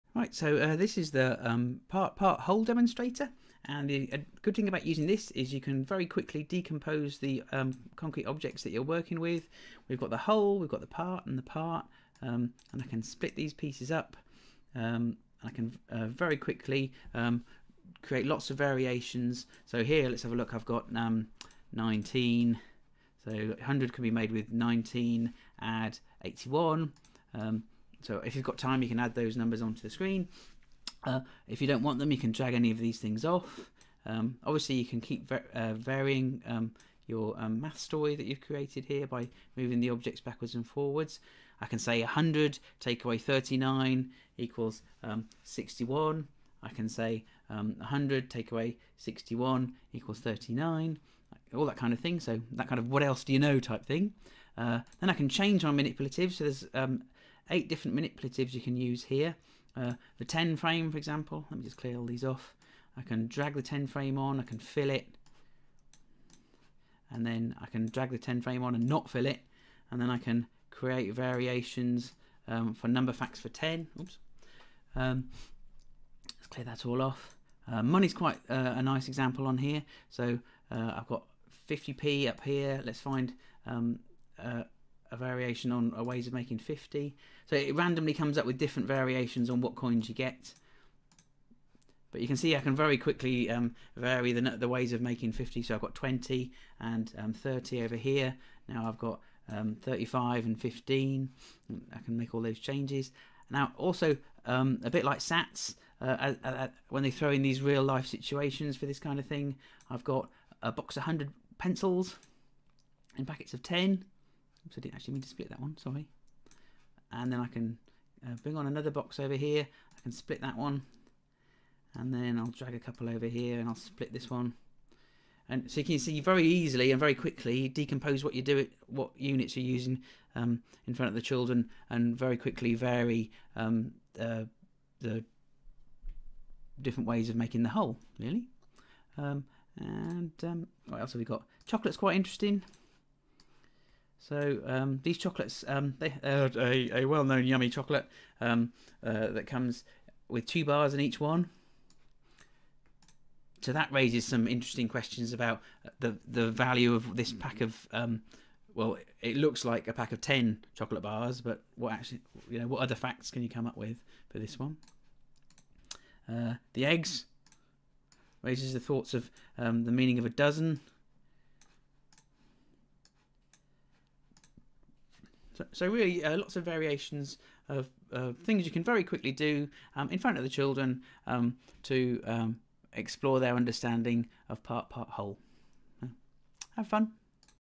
Part Part Whole
A demonstration tool designed for teachers of KS1 age children. Quickly model two parts making the whole. Easily decompose larger concrete objects.